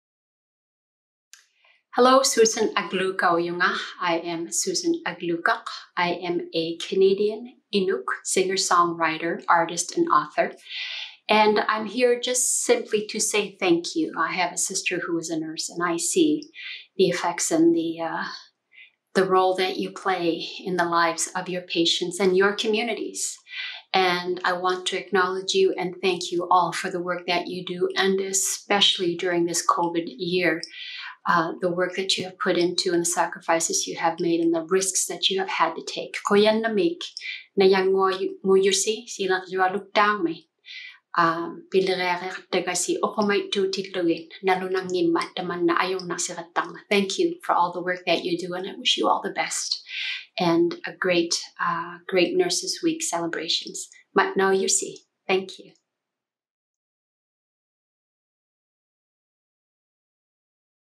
Susan Aglukark's Nurses Tribute
Canadian Inuk, singer-songwriter and author Susan Aglukark sends her heartfelt thanks to nurses!

Join us in our celebration with the Nurses Week Resiliency ReBoot to refuel hope, wellness and inspiration...